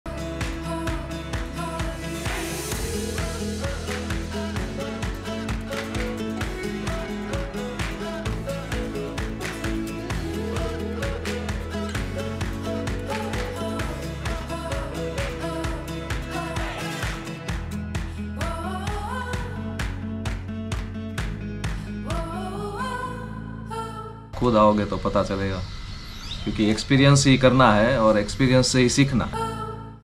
The Villagers - The Leopards - And Much More - Jawai
Get ready for a new video, till then enjoy this It's all about Jawai, The villagers and the leopards and Much More...